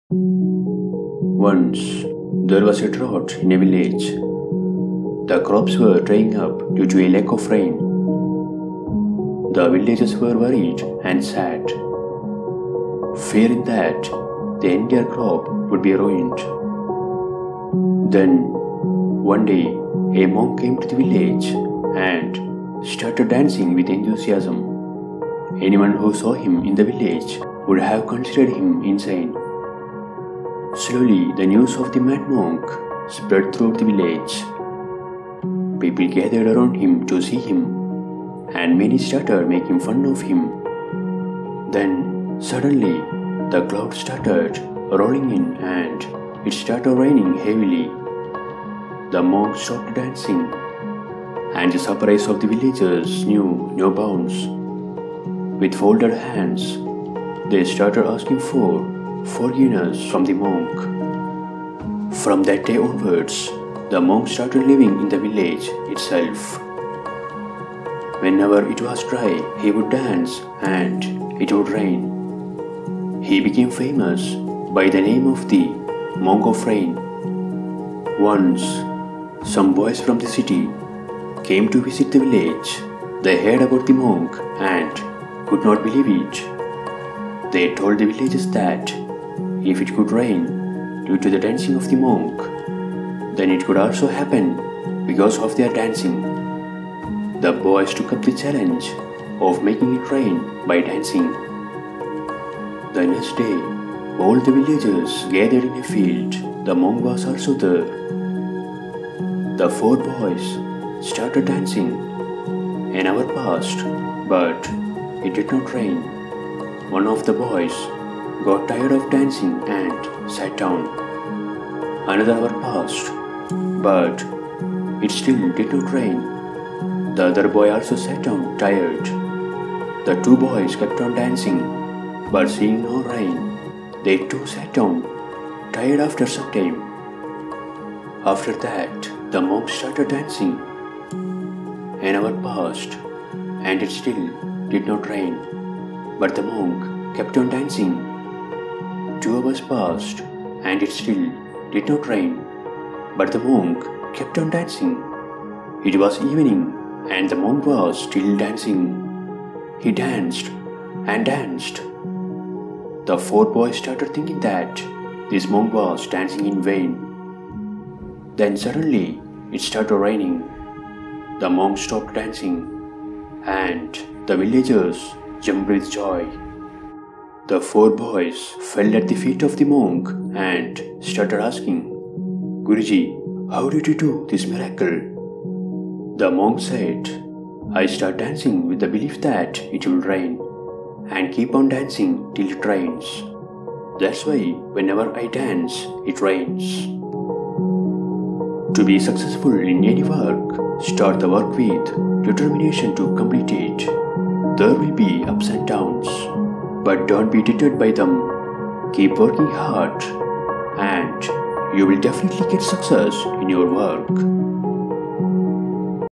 A Short Motivation For Your Life (Remember this Monk dance🔥)
This is the Motivational story of a monk. This story will inspire you a lot. This story is very interesting and also it will motivate you.

Buddhism is a religion and philosophy that originated in ancient India and is based on the teachings of Buddha. They have so many motivational and inspirational Buddhist stories that will relax our mind and also feel like meditation. To reduce stress and get mindfulness, listen to these stories.